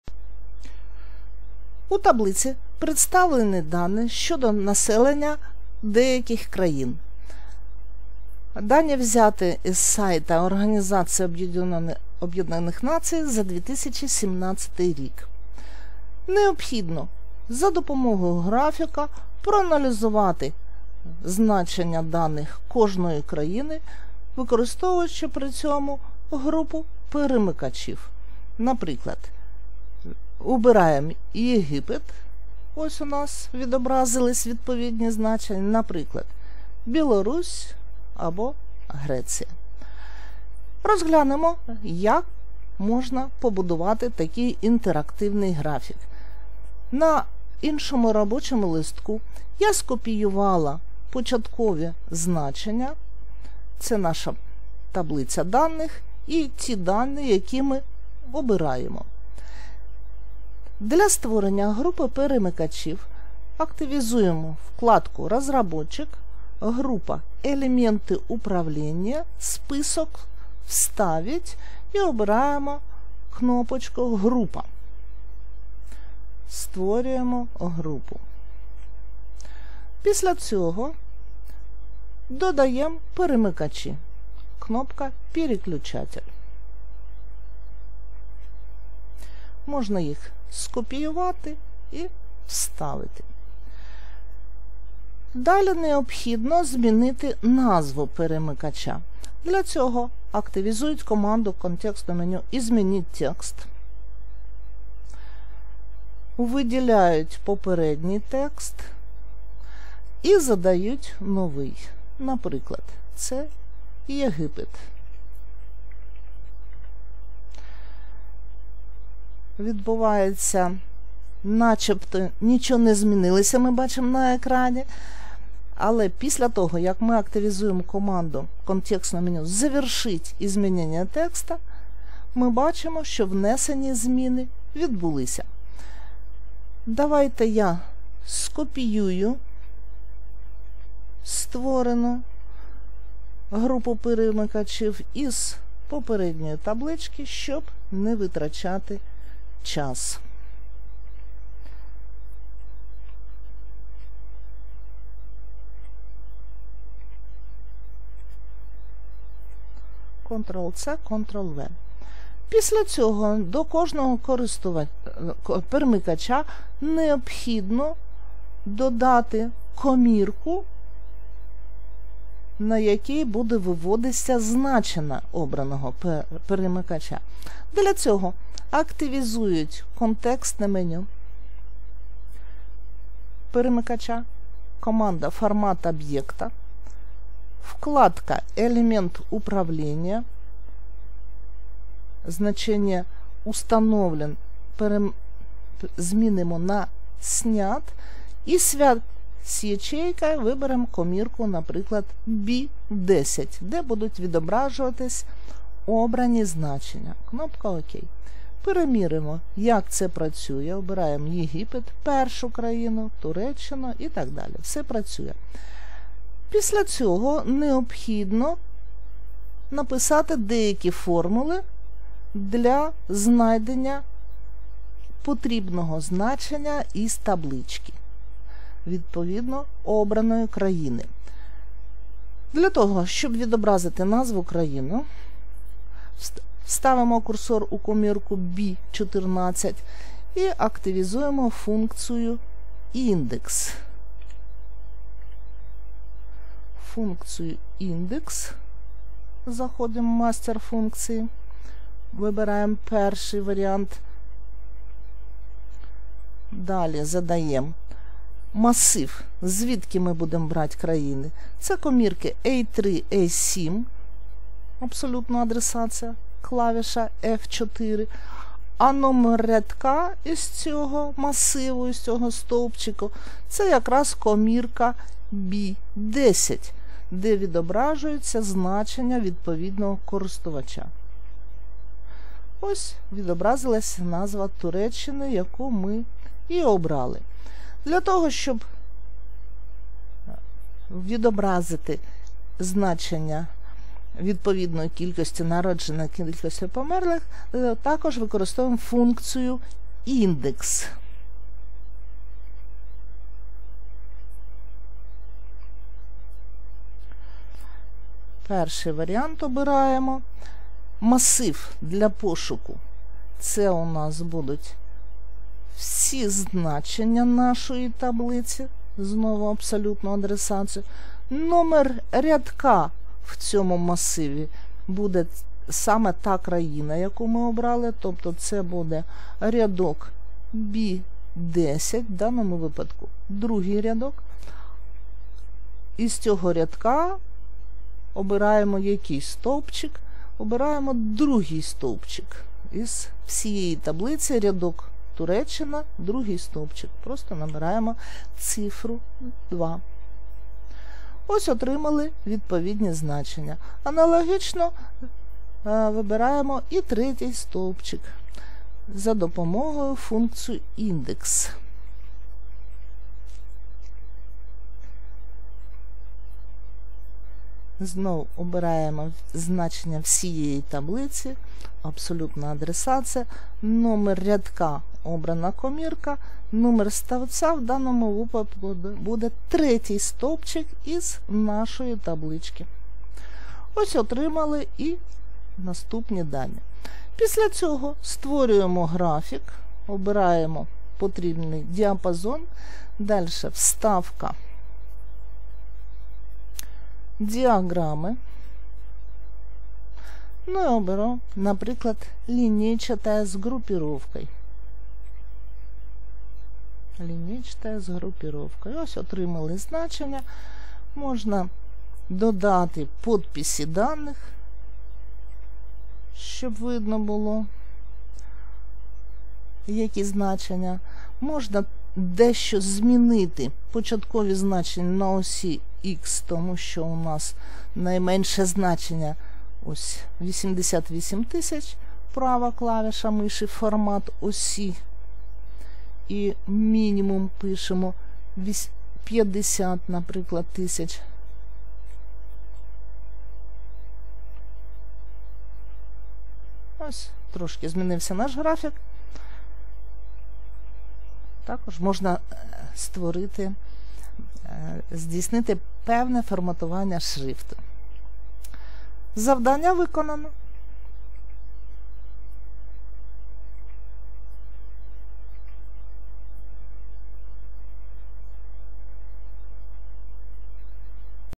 Построение диаграммы (интерактивная с выбором значений через переключатели) в MS Excel
При помощи этого видео Вы научитесь создавать интерактивные диаграммы с выбором значений при помощи группы переключателей в MS Excel